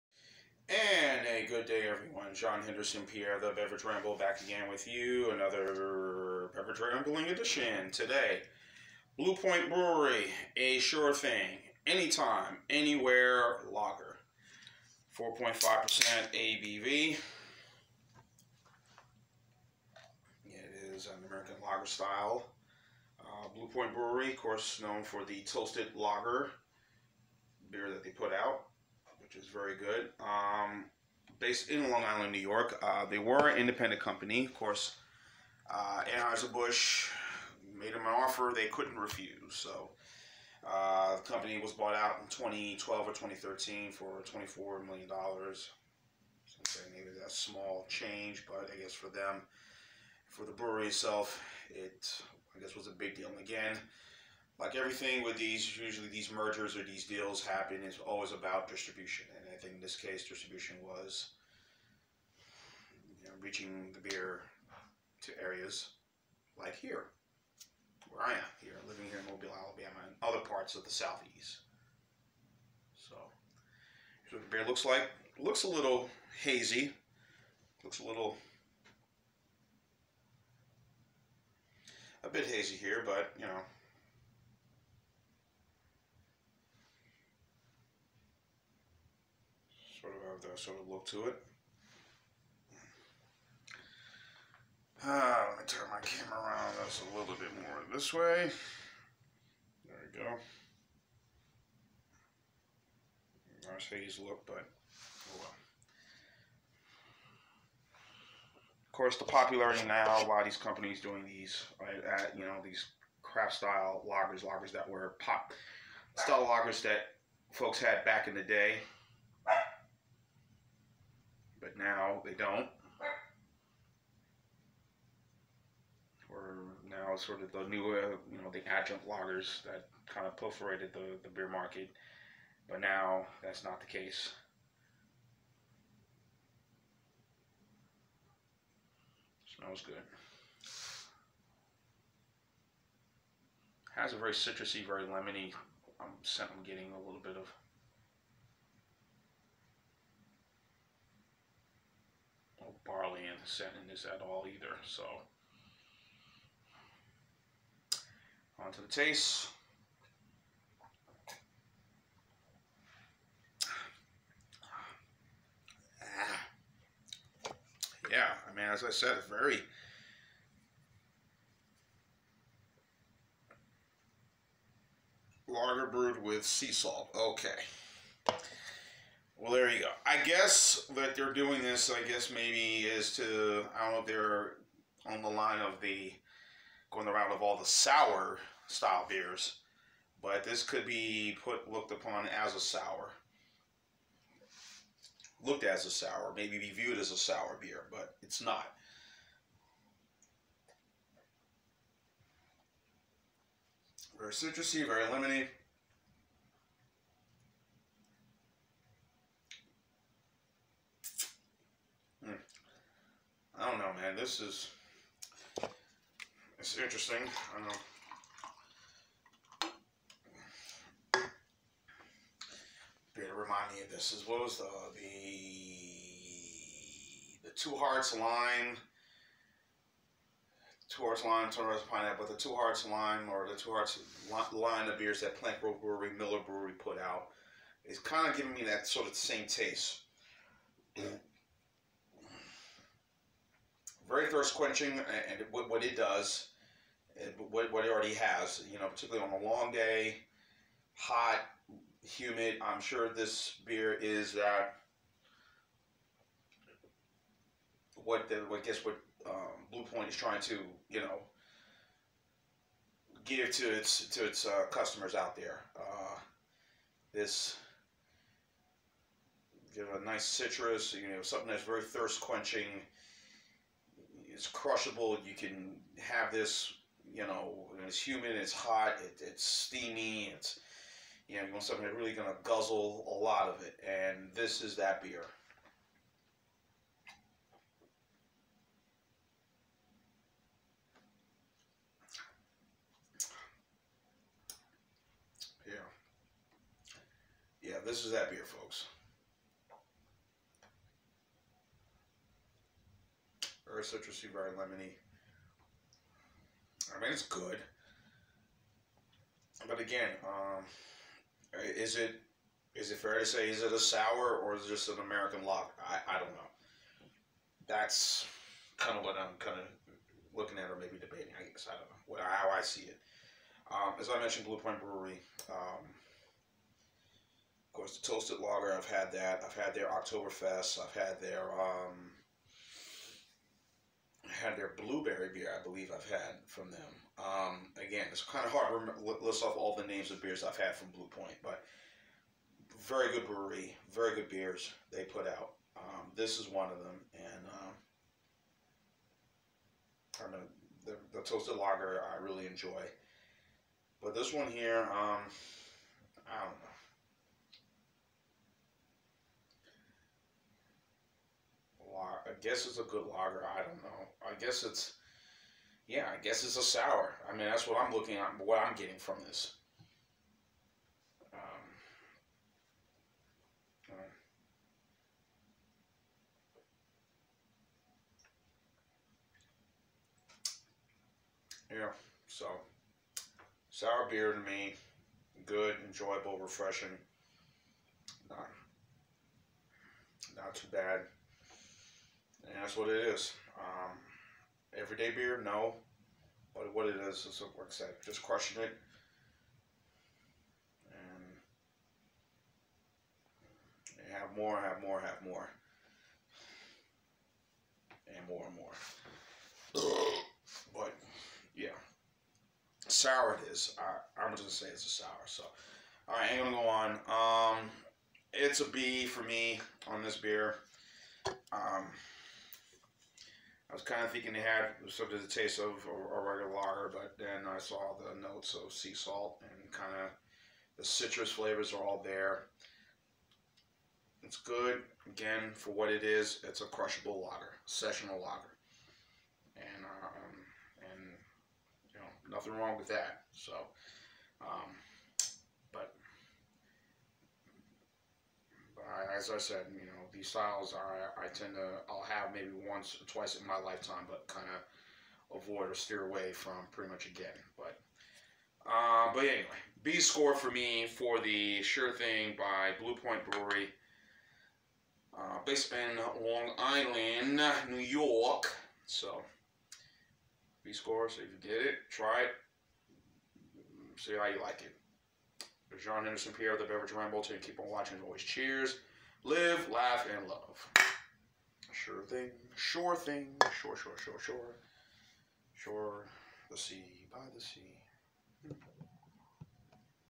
Blue Point Shore Thing Lager
An American Lager style. 4.5% ABV.

This easy-drinking, enjoyable beer has a unique tartness and citrus notes. It’s brewed with a split of Pilsner malt and wheat for a light body and a touch of creamy character. We add local sea salt from our friends at Amagansett Sea Salt Co. to the kettle for a bit of salinity.  The result is a refreshingly light beer brewed to be enjoyed Anytime, Anywhere.

Brewed by Blue Point Brewing Company. Patchogue, NY. Owned by AB-InBev. St. Louis, MO & Leuven, Belgium.